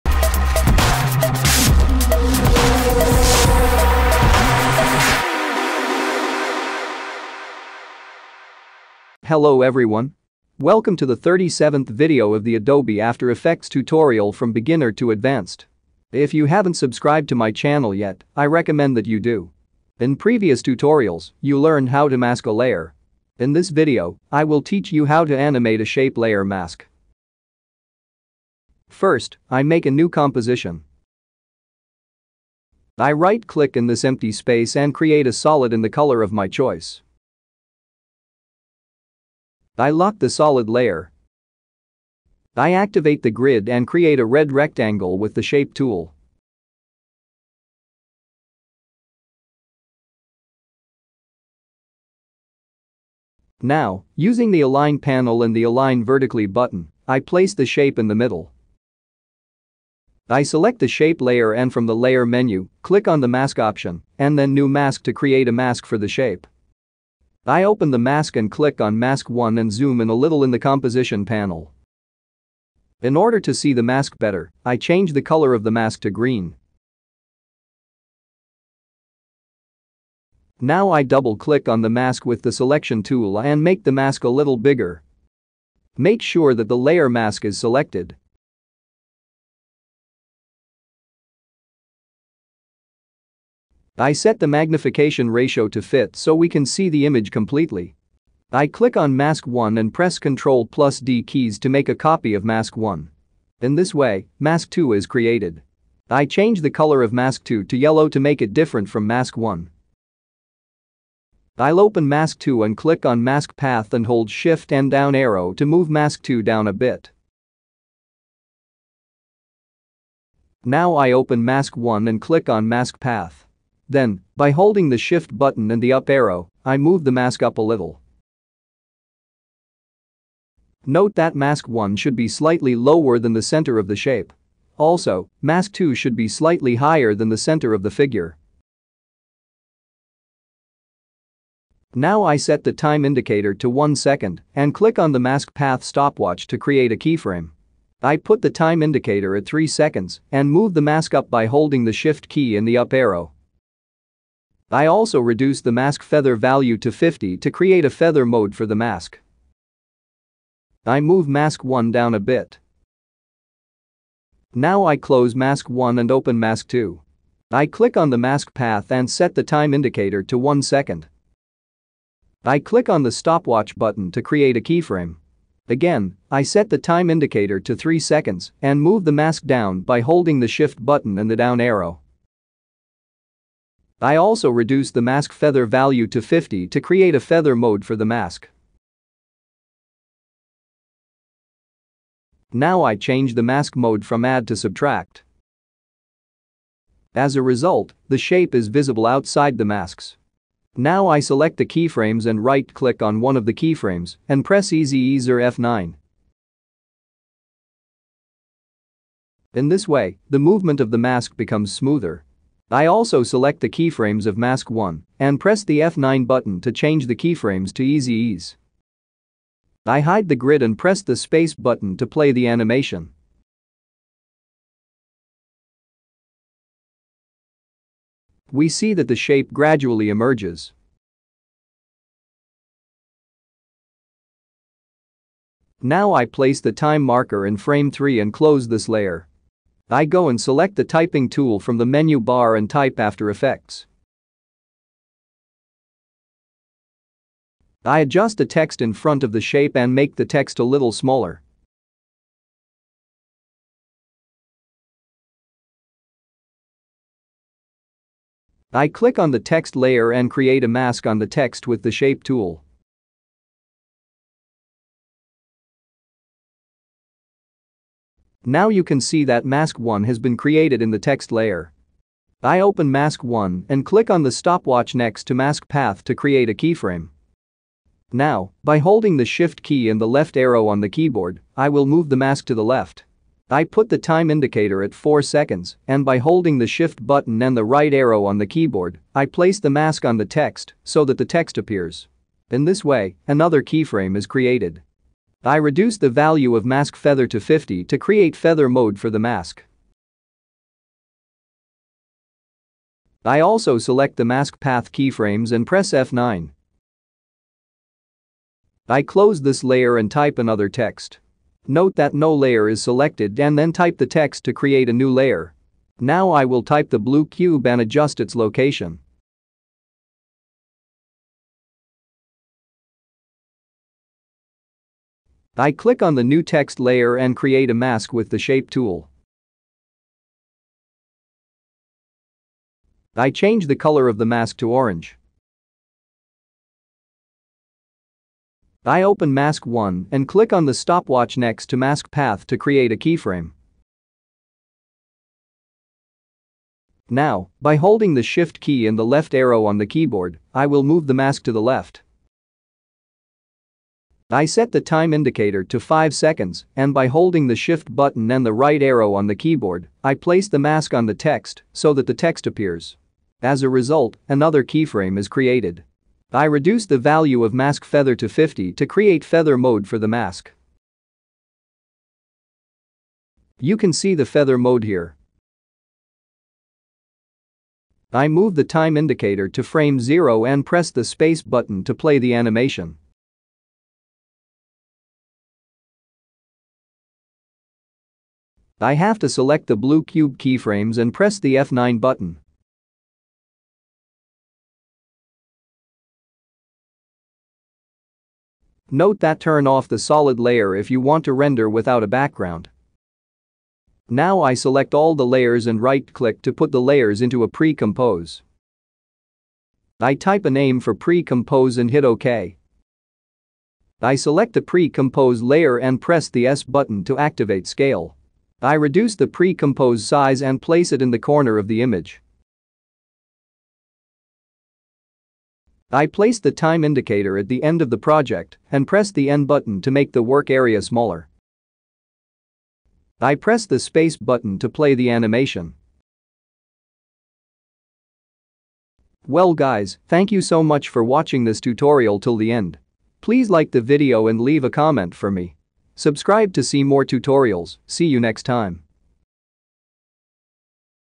Animate a Shape Layer Mask - After Effects Tutorial beginner to advanced
Adobe After Effects:  In this video, I will teach you how to animate a shape layer mask.